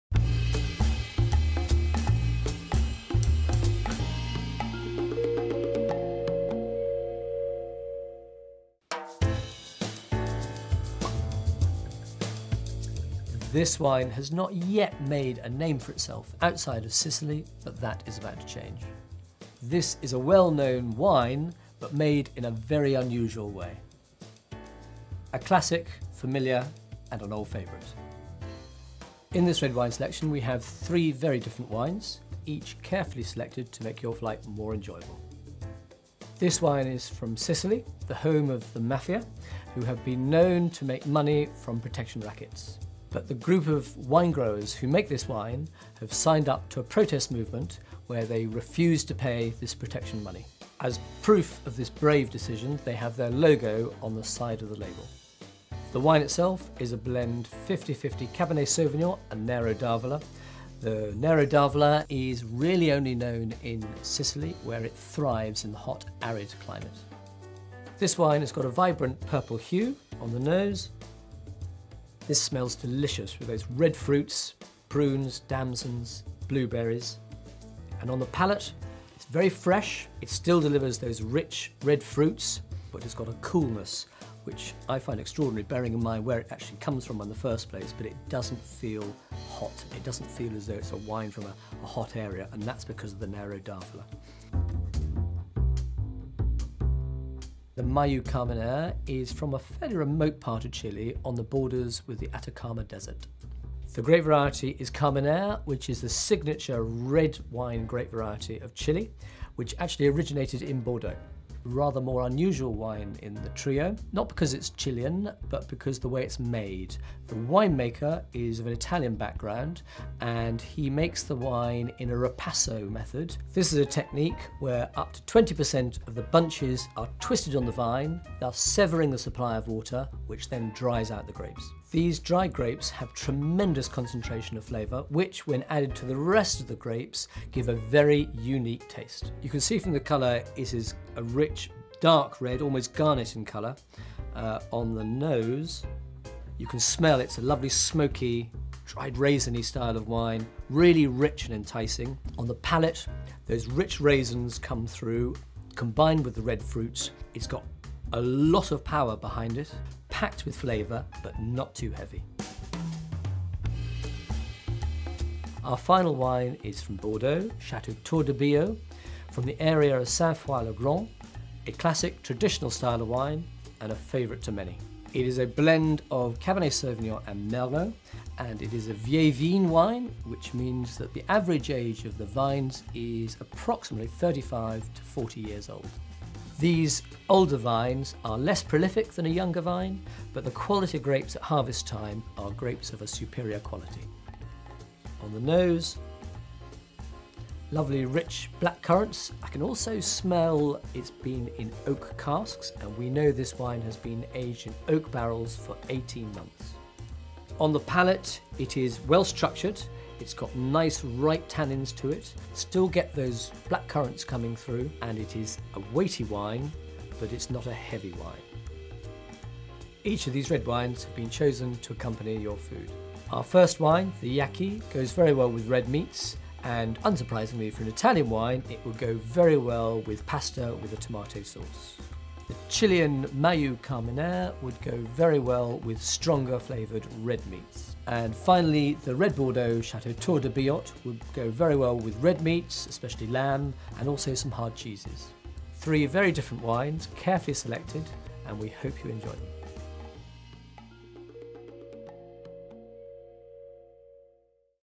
ROBERT MANNERS' RED WINE SELECTION - VIRGIN ATLANTIC
Filmed for Virgin Atlantic, in this video I talk through the Red Wine Selection offered to their Upper Class Passengers, which was available for them to watch in flight as they enjoyed the wines.